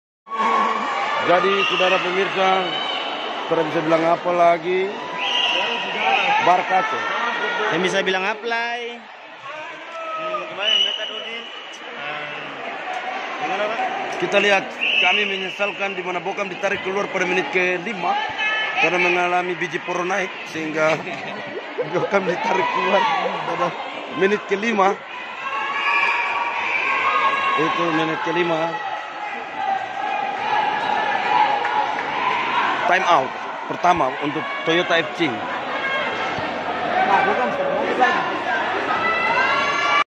DJ Sammy MR & Gang_ Broadcast Live Futsal
Vlog Pertandingan Futsal (PUMA) Putra Maibrat yang diadakan Langsung di Futsal Logam Manokwari PB🤟🤟